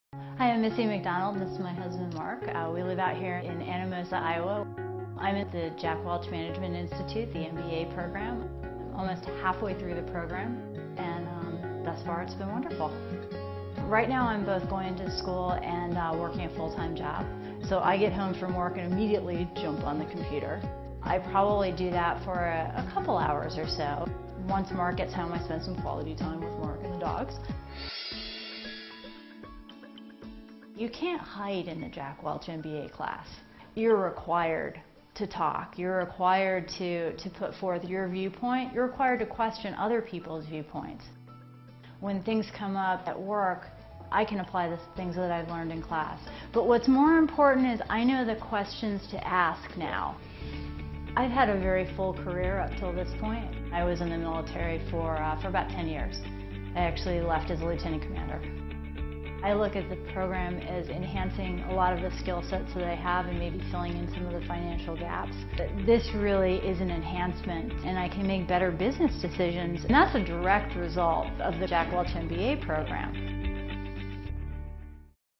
Filling In The Gaps
This former military lieutenant is making better business decisions thanks to the Jack Welch MBA program.  Hear her story.

For additional information about the Jack Welch Management Institute, please